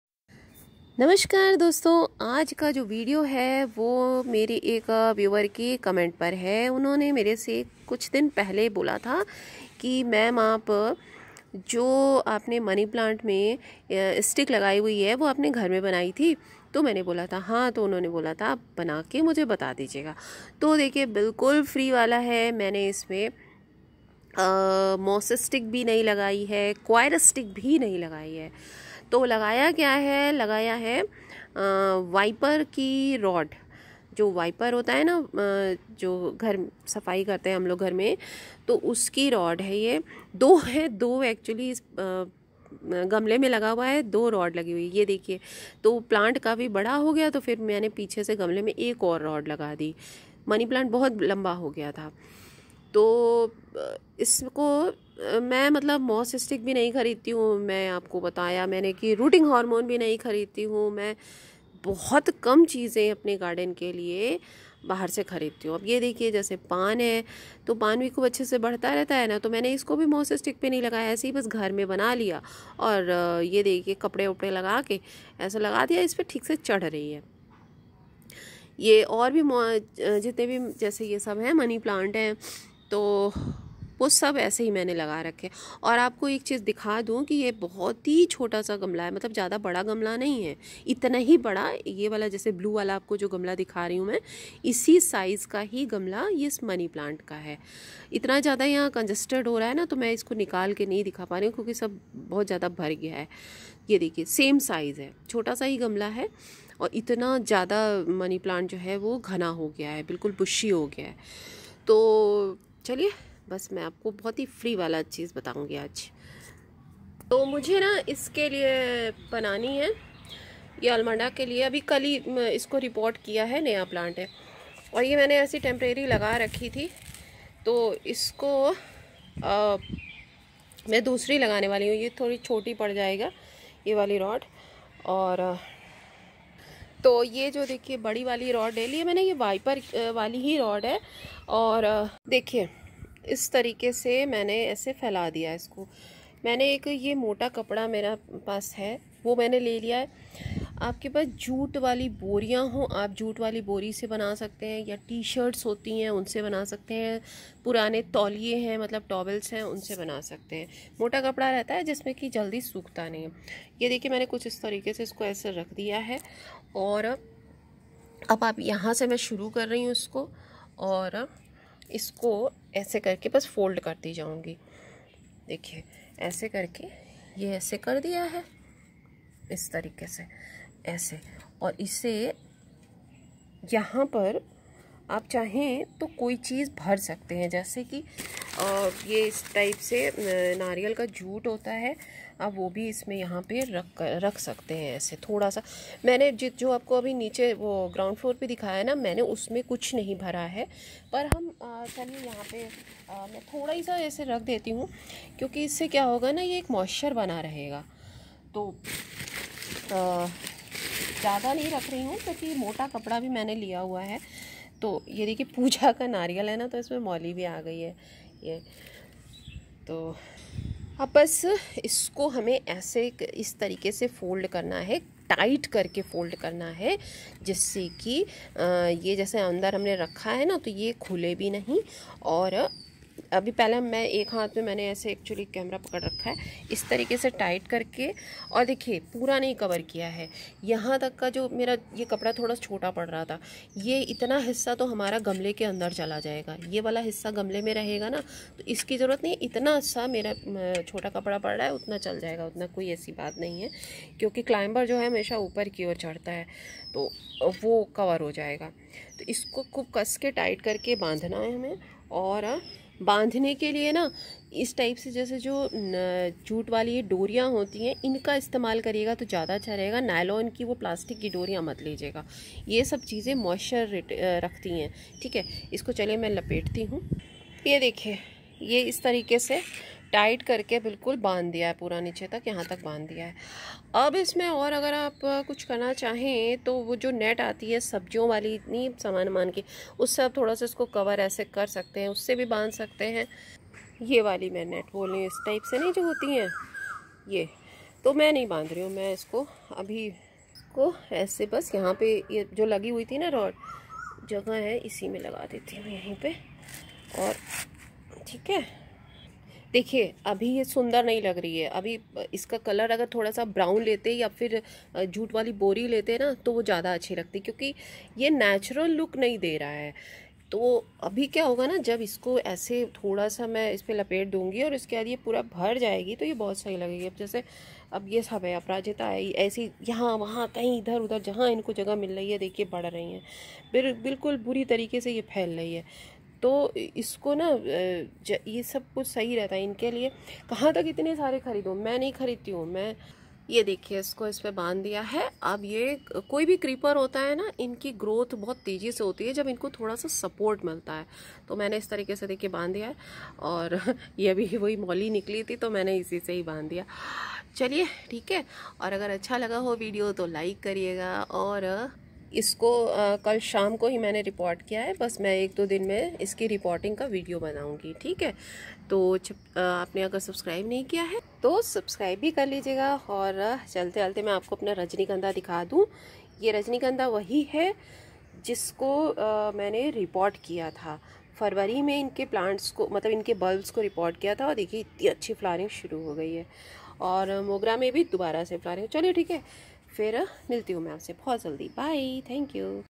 Money plant creepers के लिए खरीदती नहीं हूं Moss Coir Stick Waste pocha Rod से घर में बनाती हूं
Homemade Stick for money plant creepers of waste materials old Bekar wiper pocha Rod @OrganicGardeningForBeginners